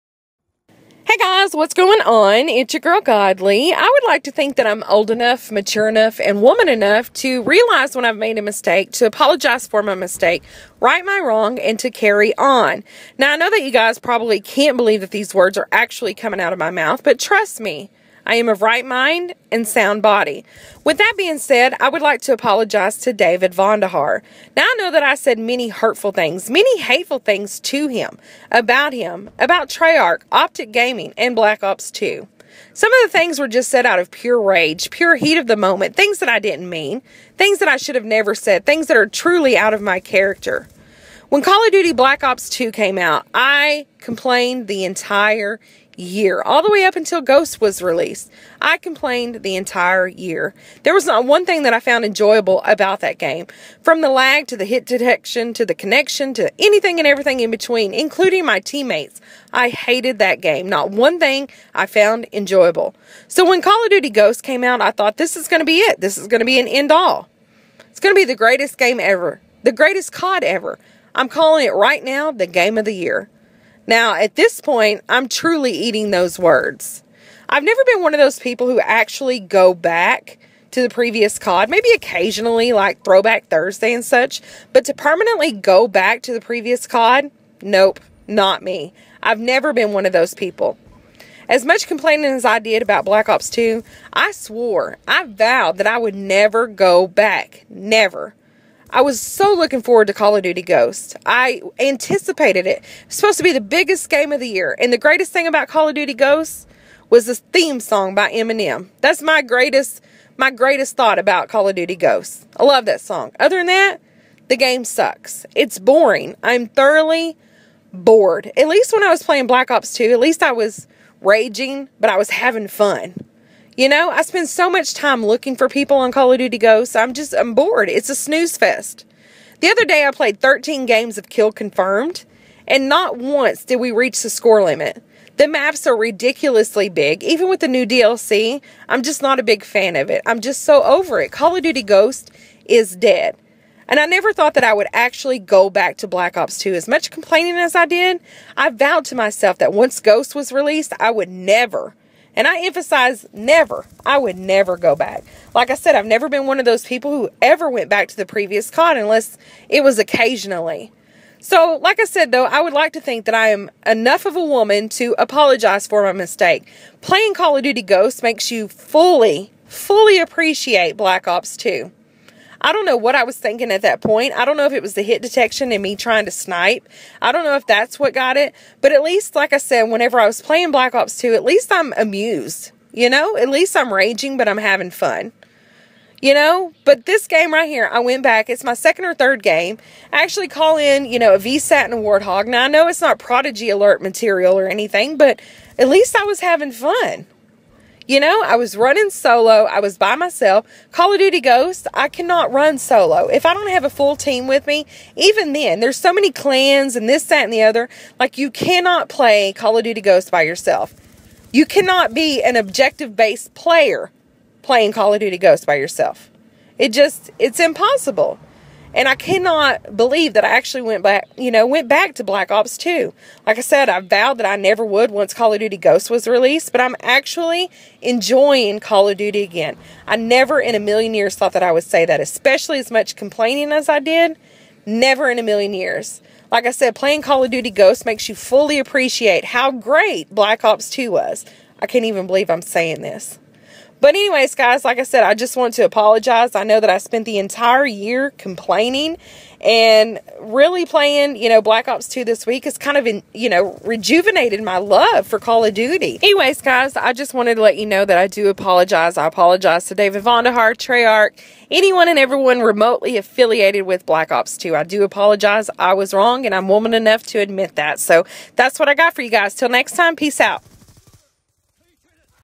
BLACK OPS 2- MY APOLOGIES @DavidVonderhaar & Treyarch
I bet that you'd never expect this from me. I was wrong and admit it. Black Ops 2 was bad but not near as bad as Ghosts. My apologies. Make sure to sub, rate, and enjoy.

follow @msgodlylocks